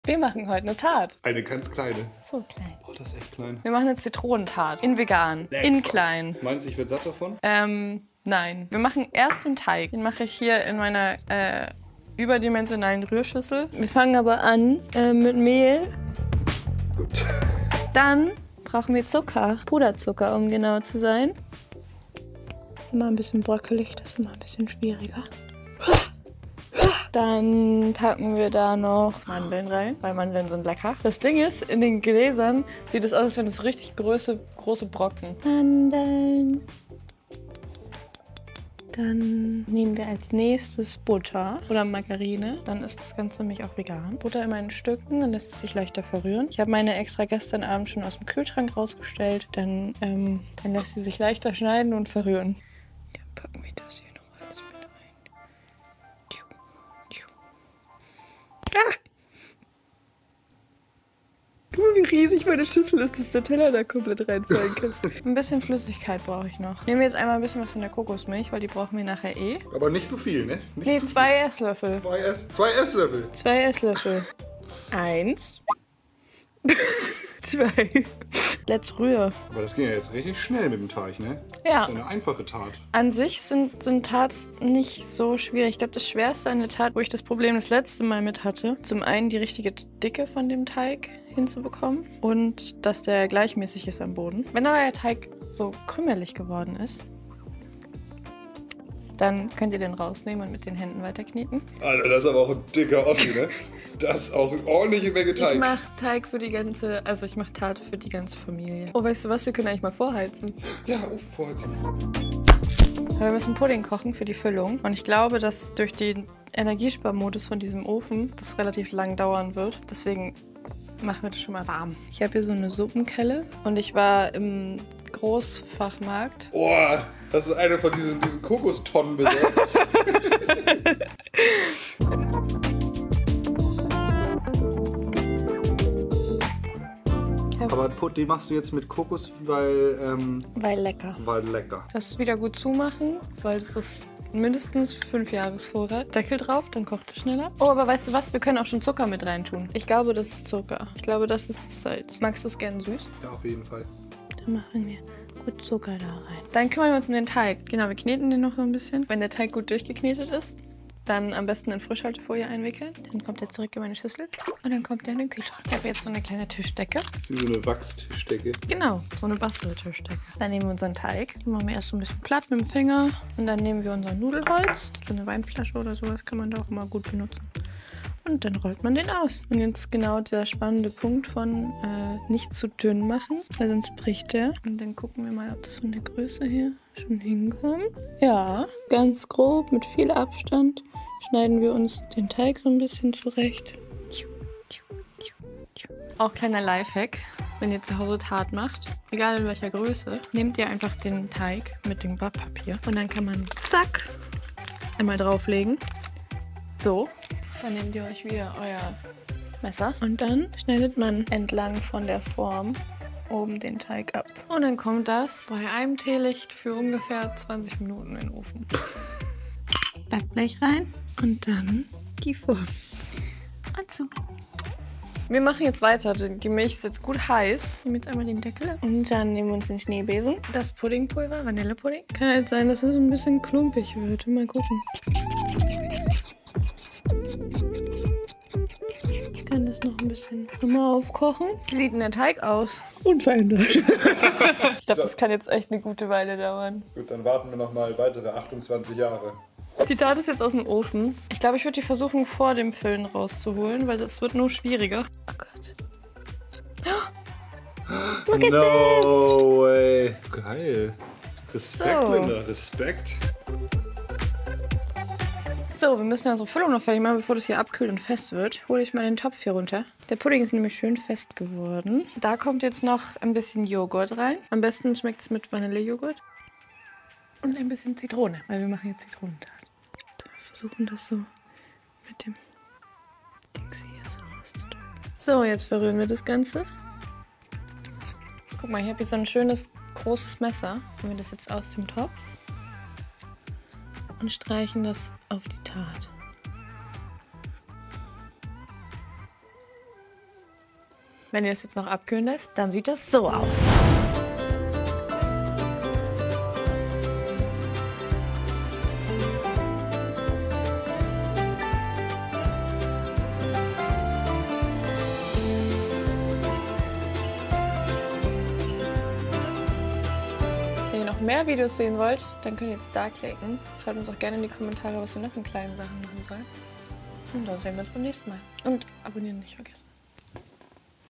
Die (vielleicht) kleinste Zitronen-Tarte der Welt
Linda hat sich in unserer winzigen "tiny kitchen" an eine Zitronen-Tarte gewagt. Ob Backen auch auf dieser nahe zu mikroskopischen Ebene funktioniert erfahrt Ihr in diesem Video!

Mein 2. Kanal Klein aber Hannah